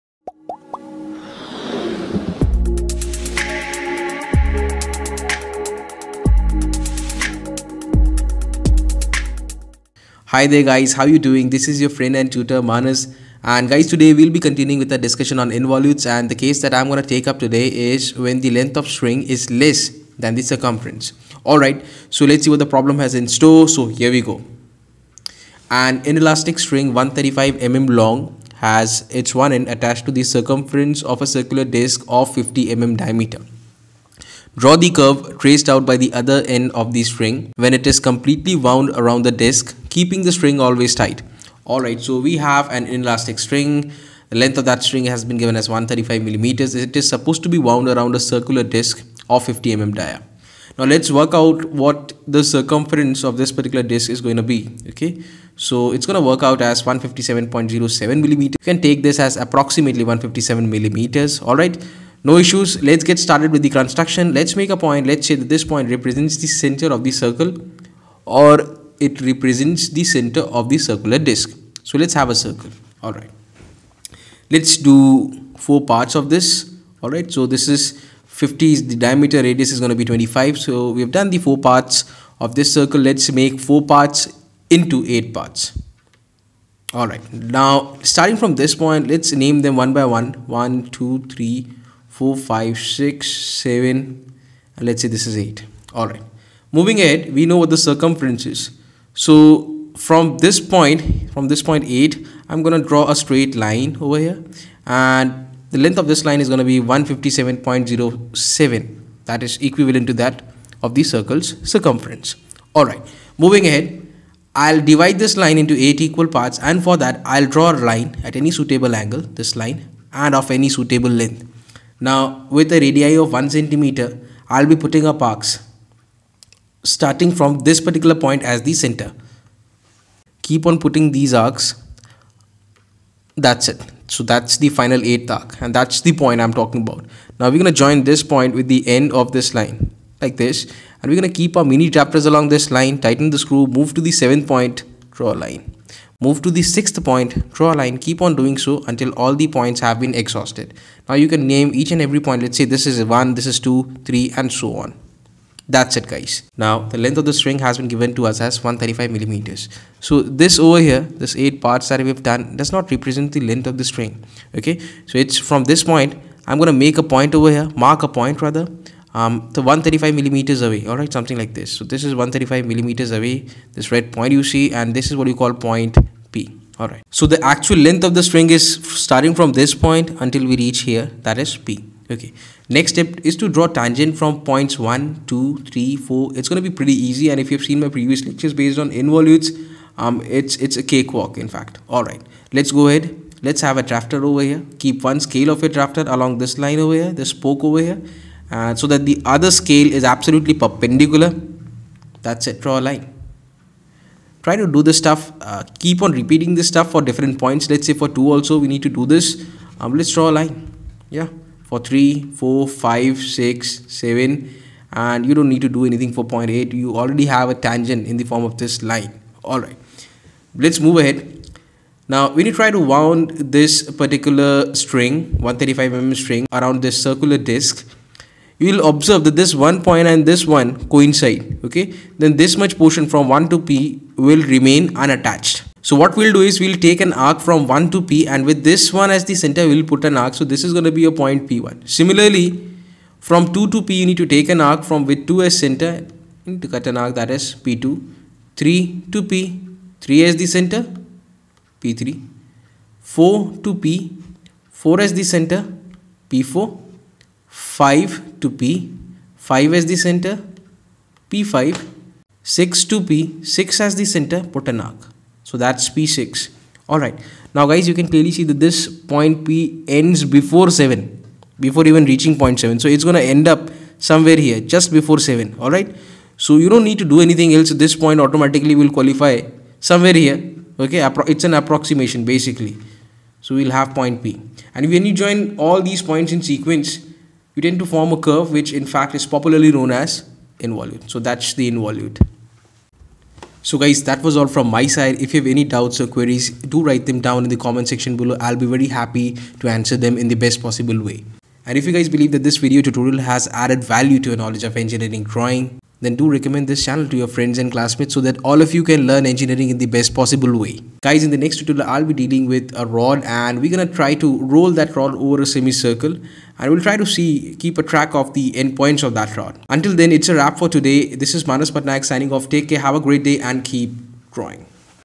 Involute_String Length is less than Circumference of circle_Problem3
Problem 3 on Involute: An inelastic string 135 mm long has its one end attached to the circumference of a circular disc of 50 mm diameter. Draw the curve traced out by the other end of the string, when it is completely wound around the disc, keeping the string always tight.